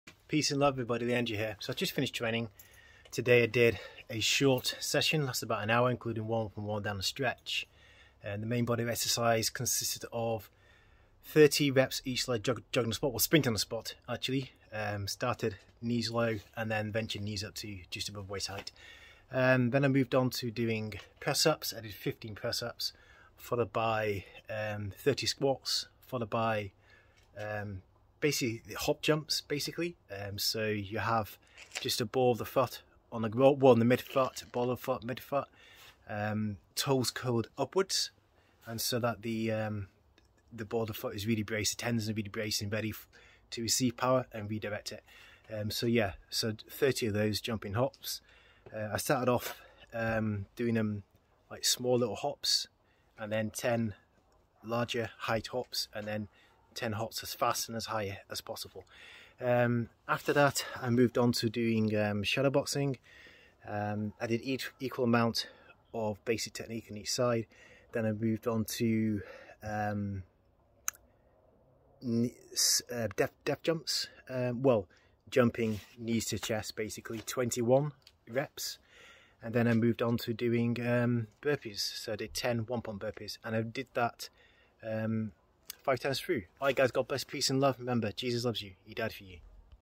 Calisthenics for Combat
5 rounds of: 30 sprint on the spot, 15 press-up,  30 squats,  30 hop jump,  shadow boxing 60s per side, 21 jump knees to chest,  10 one pump burpees.